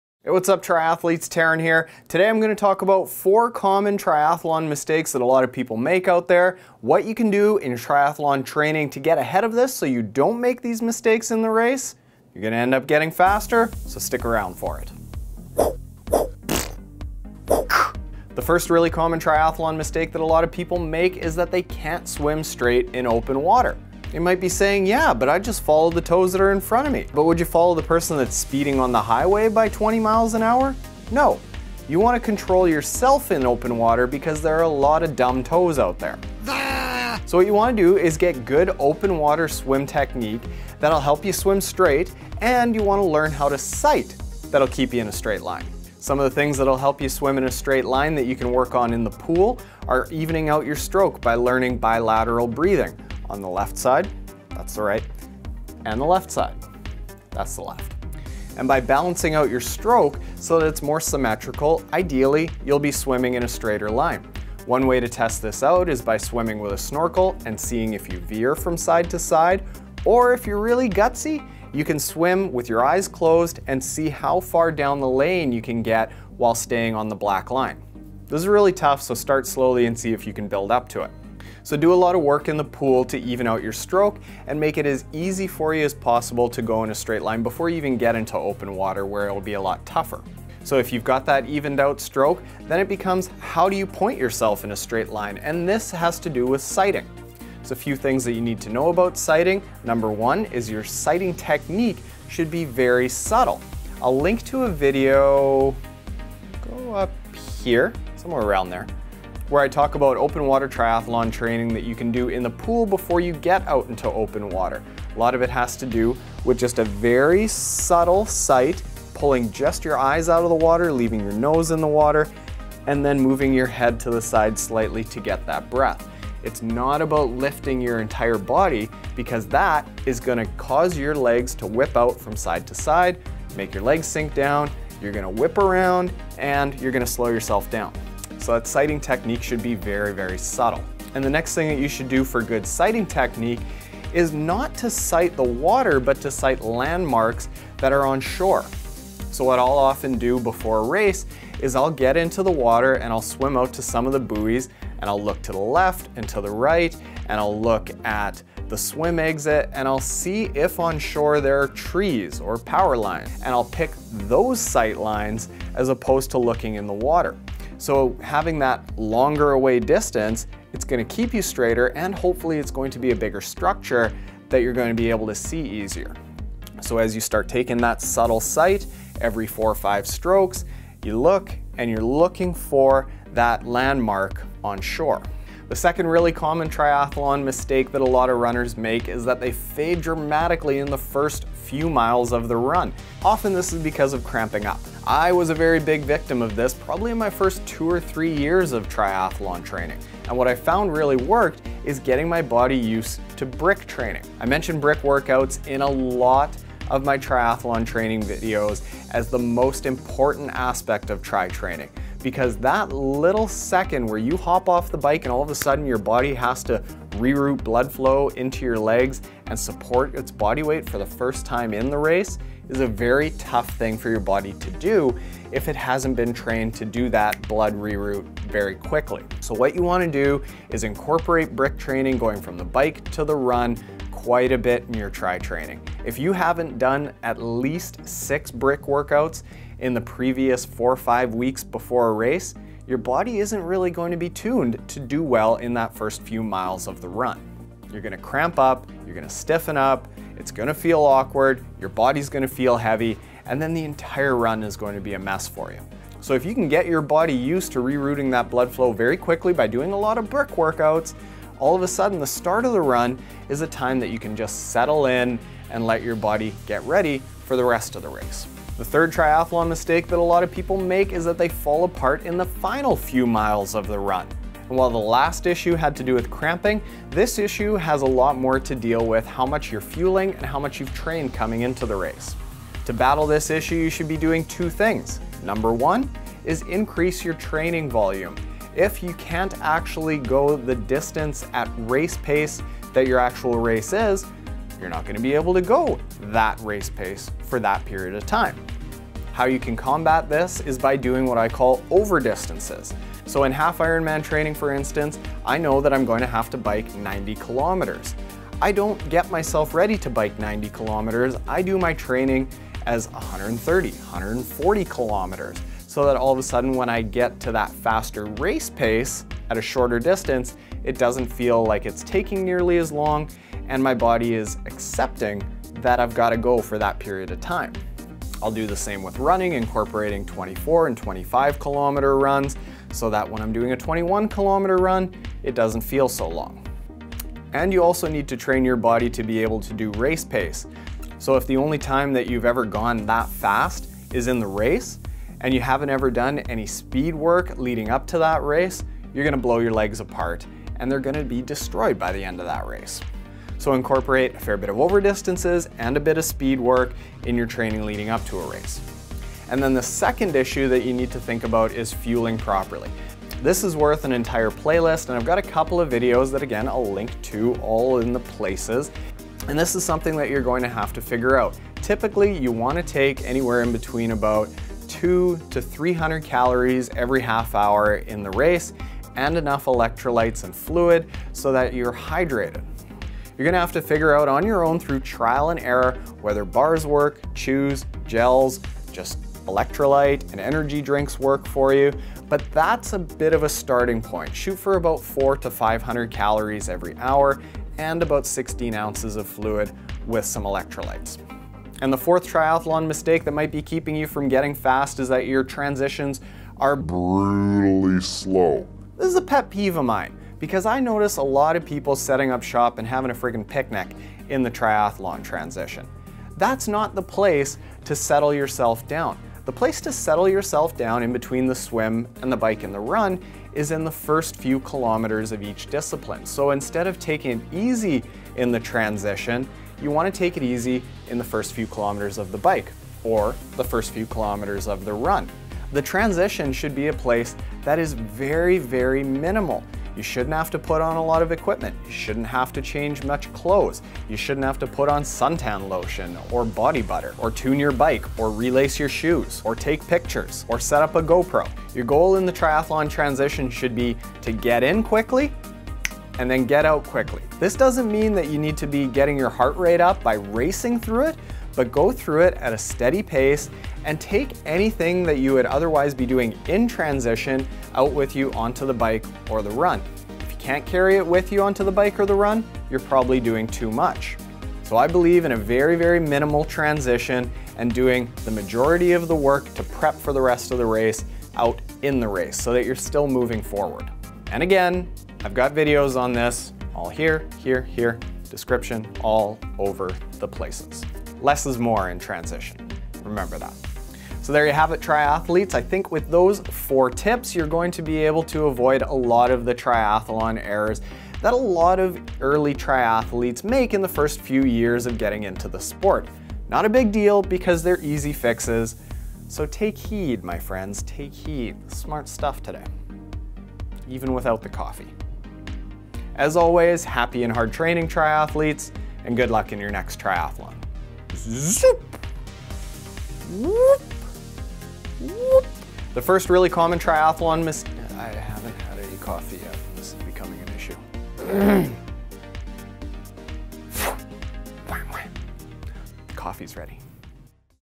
4 Triathlon Mistakes Keeping You From Getting Faster
The 4 triathlon training mistakes might be keeping you from getting faster, so watch this to make sure you're not making these triathlon training mistakes if you're a beginner triathlete.